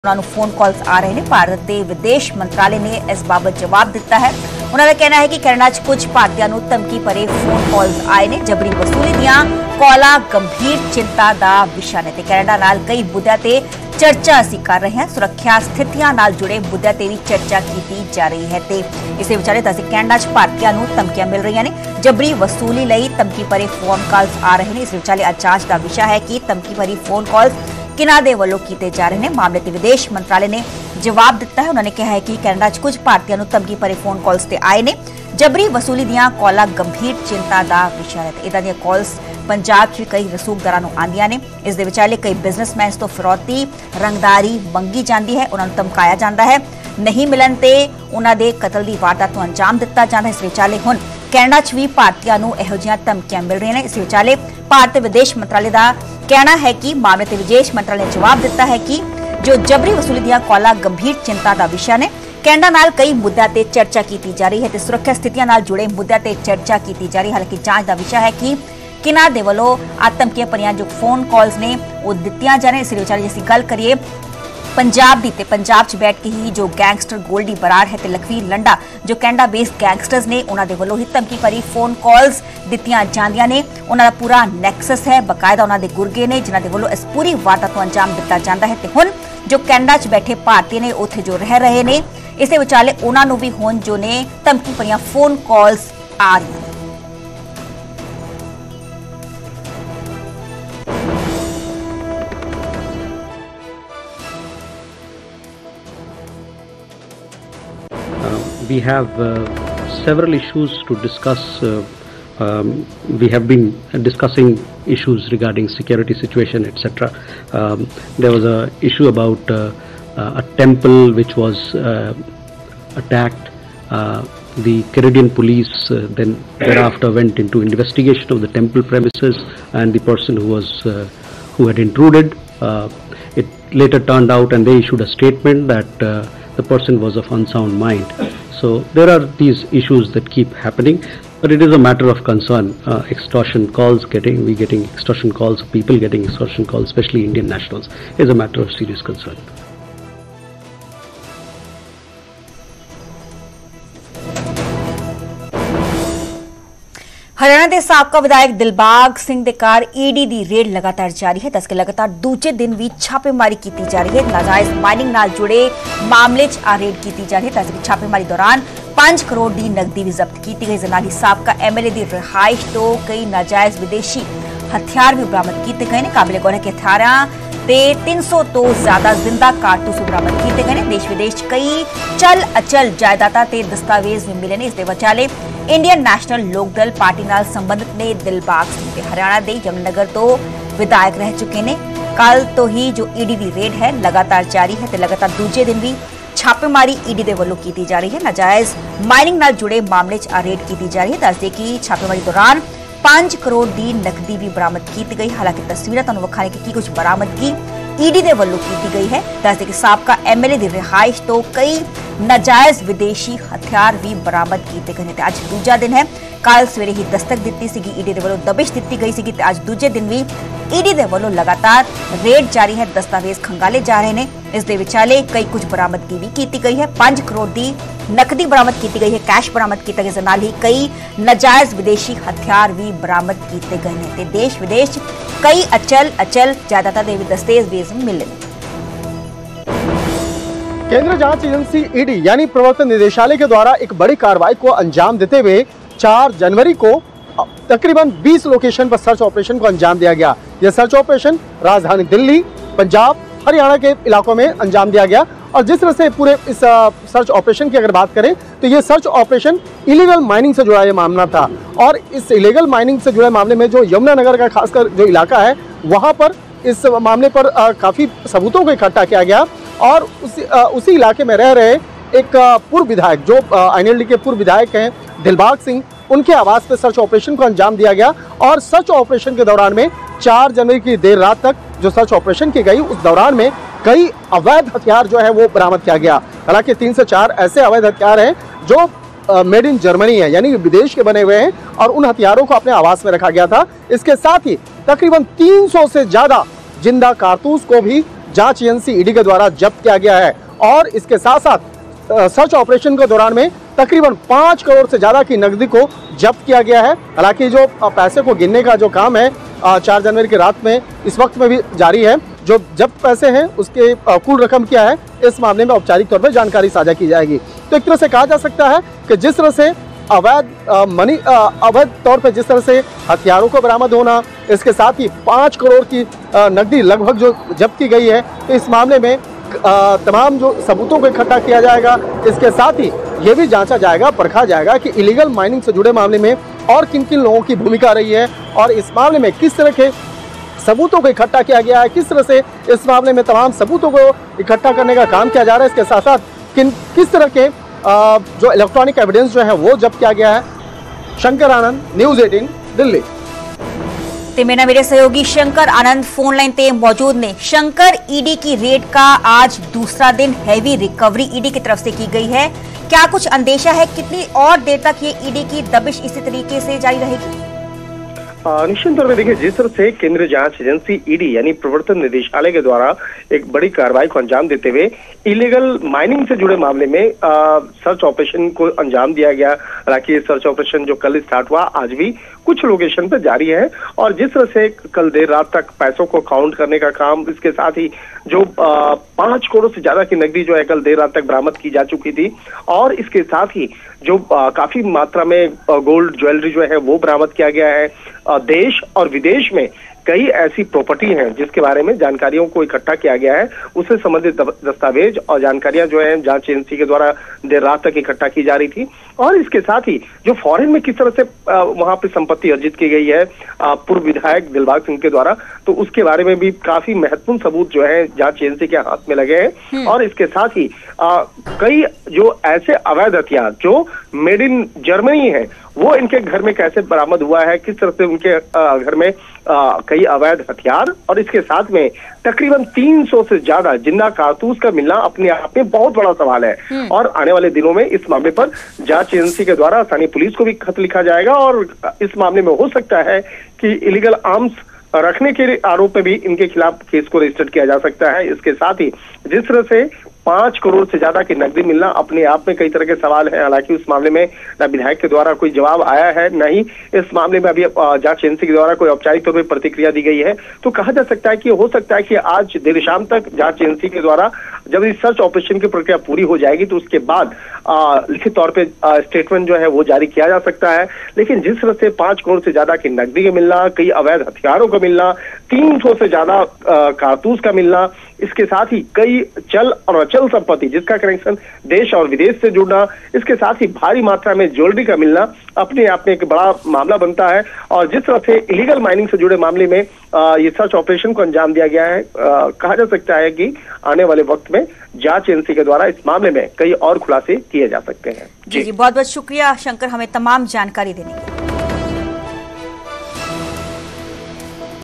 Threat to Indians in Canada | ਕੈਨੇਡਾ 'ਚ ਭਾਰਤੀਆਂ ਨੂੰ ਮਿਲ ਰਹੀਆਂ ਧਮਕੀਆਂ |MEA| Justin Trudeau | News18
ਕੈਨੇਡਾ 'ਚ ਭਾਰਤੀਆਂ ਨੂੰ ਮਿਲ ਰਹੀਆਂ ਧਮਕੀਆਂ । ਜਬਰੀ ਵਸੂਲੀ ਲਈ ਆ ਰਹੇ ਧਮਕੀ ਭਰੇ ਫੋਨ CALL। ਮਾਮਲੇ 'ਤੇ ਵਿਦੇਸ਼ ਮੰਤਰਾਲੇ ਨੇ ਦਿੱਤਾ ਜਵਾਬ। 'ਕੈਨੇਡਾ 'ਚ ਕੁਝ ਭਾਰਤੀਆਂ ਨੂੰ ਧਮਕੀ ਭਰੇ ਫੋਨ CALL ਆਏ'। 'ਜਬਰੀ ਵਸੂਲੀ ਦੀਆਂ ਕਾਲਾਂ ਗੰਭੀਰ ਚਿੰਤਾ ਦਾ ਵਿਸ਼ਾ'। 'ਕੈਨੇਡਾ ਨਾਲ ਕਈ ਮੁੱਦਿਆਂ 'ਤੇ ਚਰਚਾ ਕਰ ਰਹੇ ਹਾਂ'। 'ਸੁਰੱਖਿਆ ਸਥਿਤੀਆਂ ਨਾਲ ਜੁੜੇ ਮੁੱਦਿਆਂ 'ਤੇ ਚਰਚਾ ਜਾਰੀ'